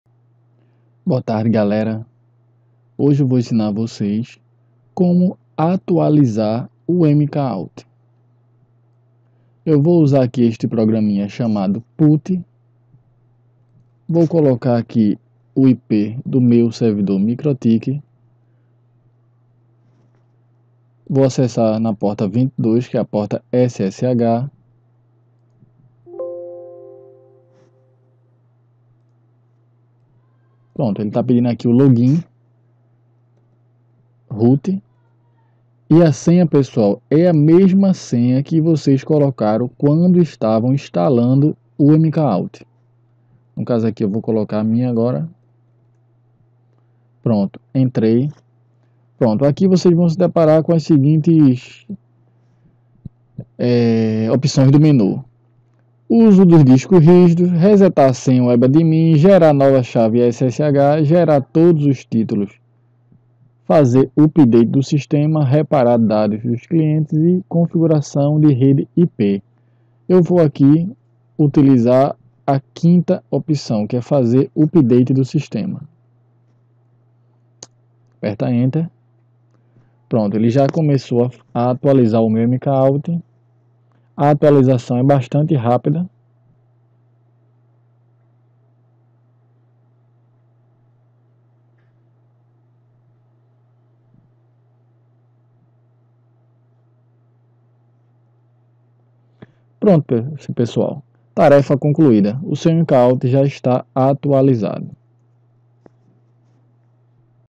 Atualizar Mk-auth via SSH
Neste video eu ensino a atualizar o Mk-auth da maneira mais facil possivel.